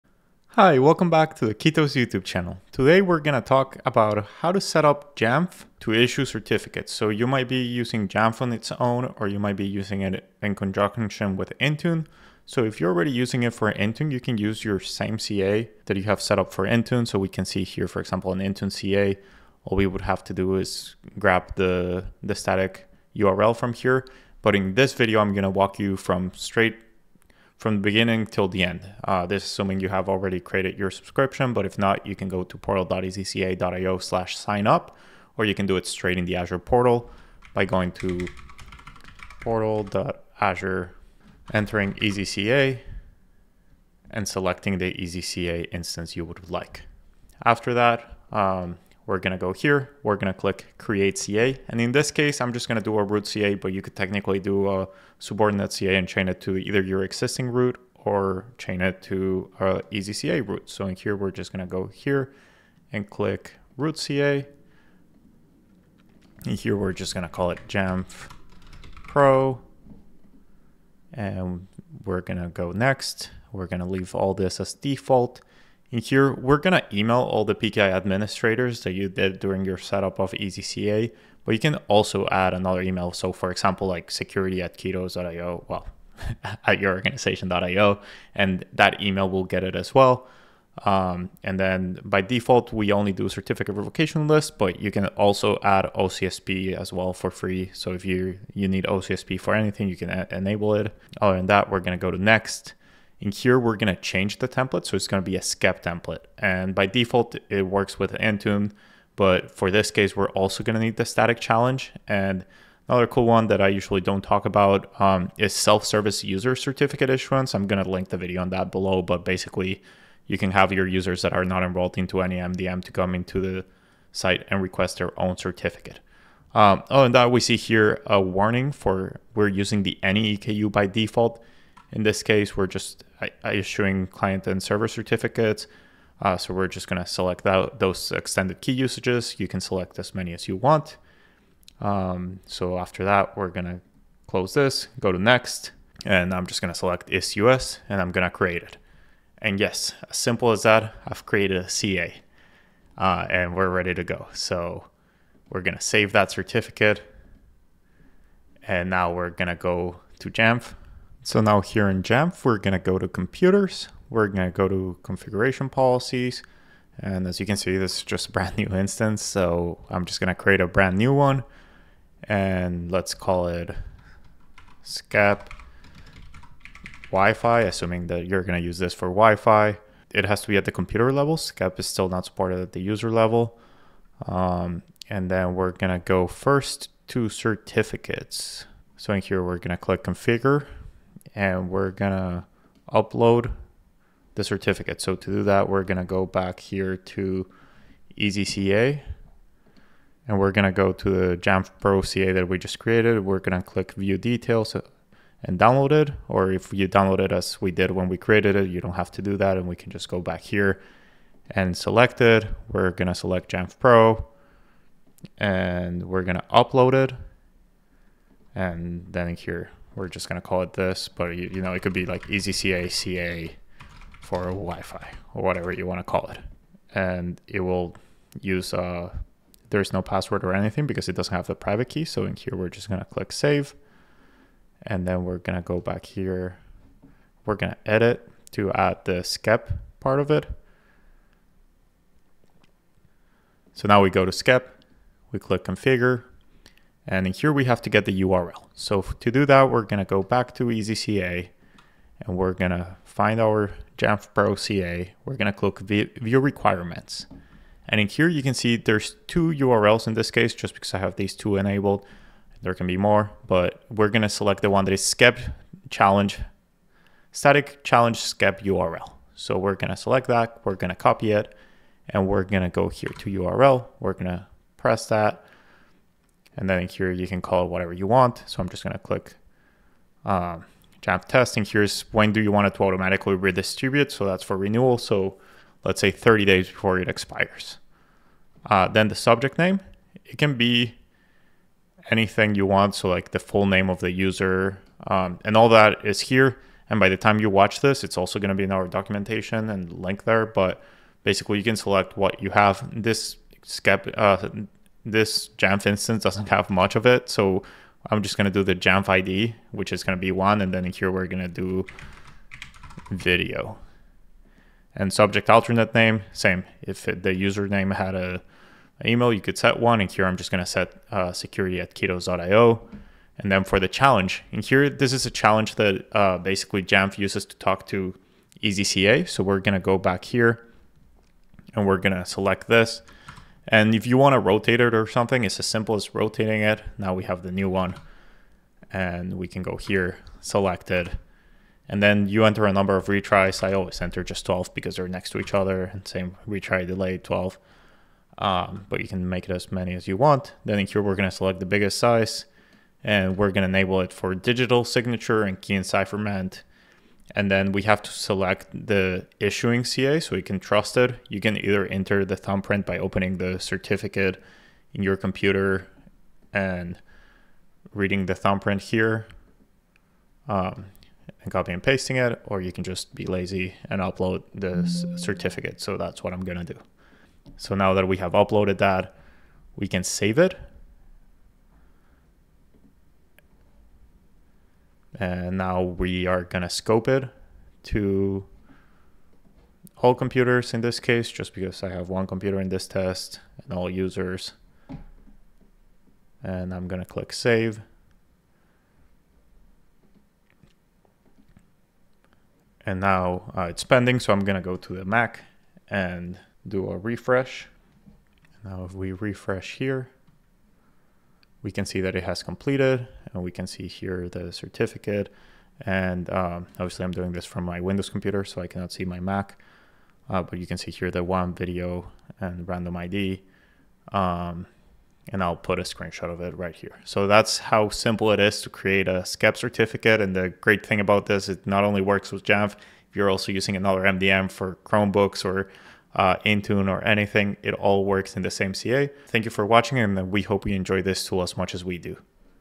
How to Create a Cloud Based Certificate Authority for Jamf Pro and Intune
In This video we go over how to create a Cloud Based PKI /Certificate Authority for SCEP certificate distribution through JAMF Pro and Intune or any other MDM you might be using.